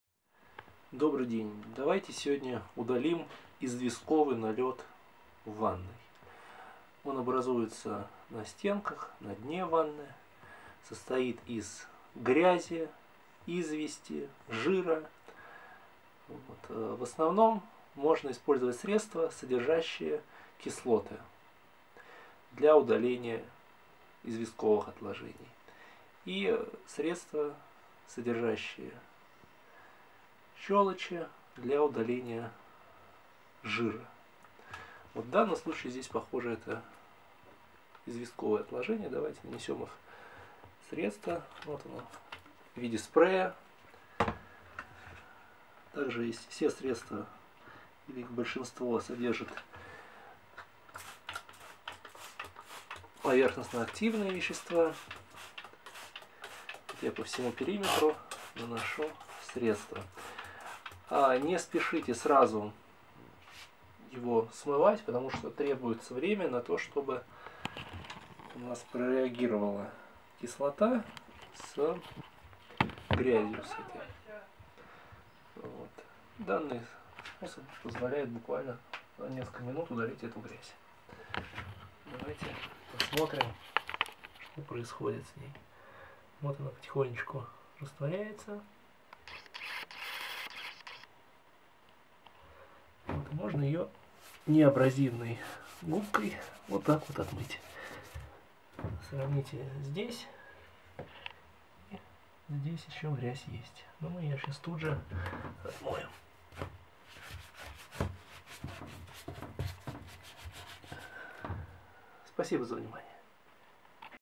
Как отмыть ванну от желтого налета
Для отмывания ванны от желтого налета используйте микрофибровую тряпку, смоченную моющим средством с поверхностно-активными веществами (ПАВ), далее с кислотами, далее с активным кислородом, и, наконец, с хлором.